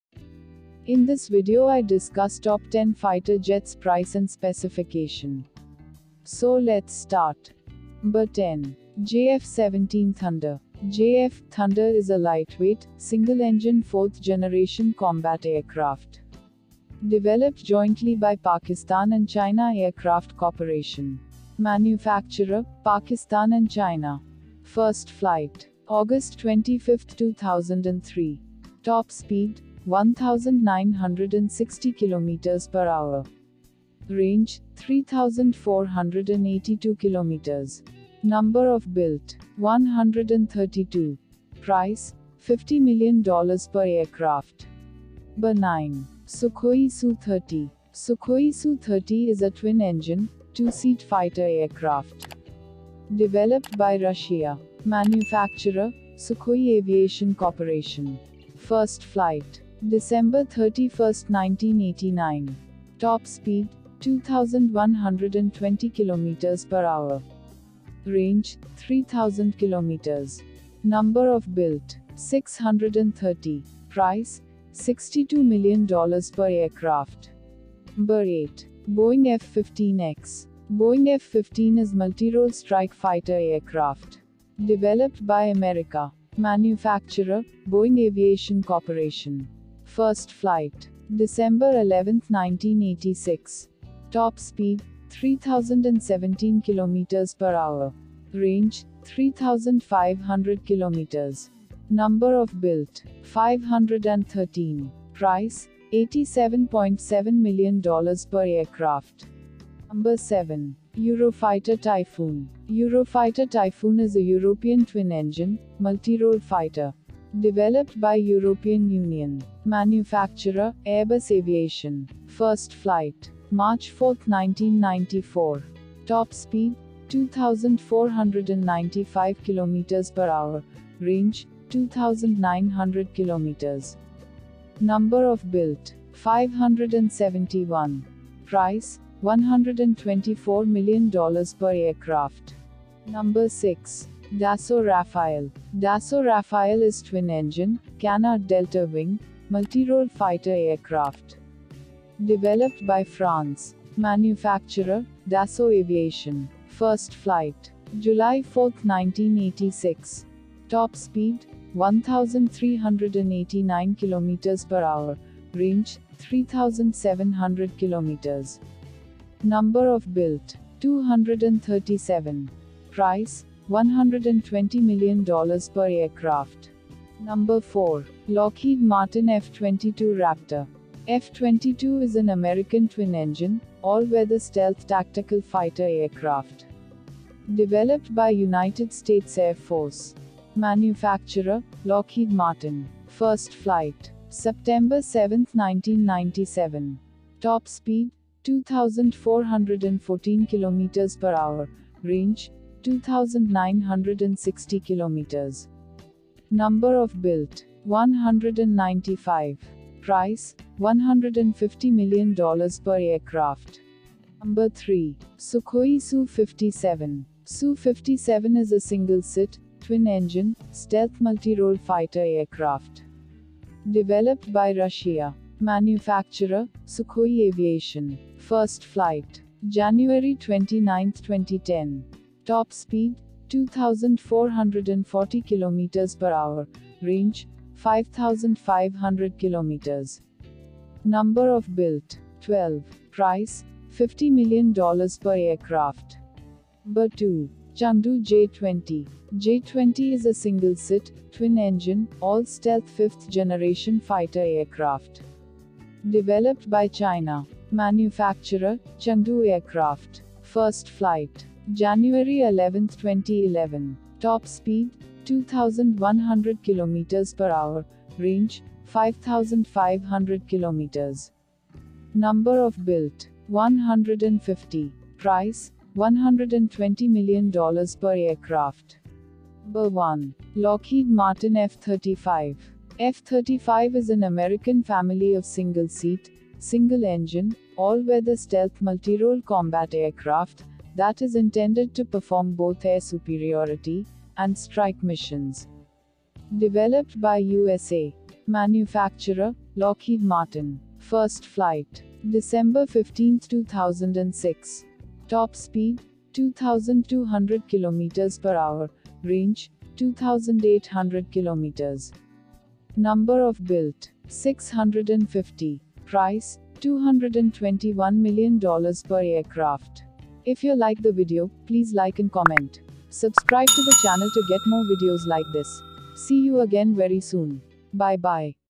Top 10 best Fighter Jets in the world - Price & Specification | Best Fighter Planes | F 35, Boeing
Best 10 fighter jets in the world. F -35, Chengdu J 20, Sukhoi su 57, Lockheed Martin F 22 Rapter, Chengdu fc 31, Dassault Rafale, Eurofighter Typhoon, Boeing f 15 ex, Sukhoi su 30 and JF - 17 Thunder.

US fighter jets vs Chinese fighter jets vs Russian fighter jets. This video compare  top 10 fighter jets price and specification.

f-35

Top 10Fighter Jets Price and specification

Number 10. JF - 17 Thunder.
JF - Thunder is a lightweight, single engine fourth generation combat aircraft. Developed jointly by Pakistan and china aircraft corporation.

Manufacturer. - Pakistan & China
First flight. - August 25, 2003.
Top Speed. - 1,960 km per hour.
Range. - 3,482 km.
Number of built. - 132.
Price. - $50 million per aircraft.

Number 9. Sukhoi su - 30.
Sukhoi su 30 is a twin engine, two seat fighter aircraft. Developed by Russia.

Manufacturer. - Sukhoi Aviation Corporation.
First flight. - December 31, 1989.
Top Speed. - 2,120 km per hour.
Range. - 3,000 km.
Number of built. - 630.
Price. - $62 million per aircraft.

Number 8. Boeing F - 15 EX
Boeing f 15 is multirole strike fighter aircraft. Developed by America.

Manufacturer. - Boeing Aviation Corporation.
First flight. - December 11, 1986.
Top Speed. - 3,017 km per hour.
Range. - 3,500 km.
Number of built. - 513.
Price. - $87.7 million per aircraft.

Number 7. Eurofighter Typhoon.
Eurofighter Typhoon is a European twin engine, multirole fighter. Developed by European union.

Manufacturer. - Airbus Aviation.
First flight. - March 4, 1994.
Number of built. - 571.
Price. - $124 million per aircraft.

Number 6. Dassault Rafael.
Dassault Rafael is twin engine, canard delta wing, multirole fighter aircraft. Developed by France.

Manufacturer. - Dassault Aviation.
First flight. - July 4, 1986.
Number of built. - 237.
Price. - $120 million per aircraft.

Number 5. Shenyang FC - 31.
Shenyang fc 31 is twin engine, mid size fifth generation fighter aircraft.
Developed by China.

Manufacturer. - Shenyang Aviation Corporation.
First flight. - October 31, 2012.
Number of built. - Prototype.
Price. - $70 million per aircraft.

4. Lockheed Martin F 22 Rapter
3. Sukhoi su 57
2. Chengdu J 20
1. Lockheed Martin F 35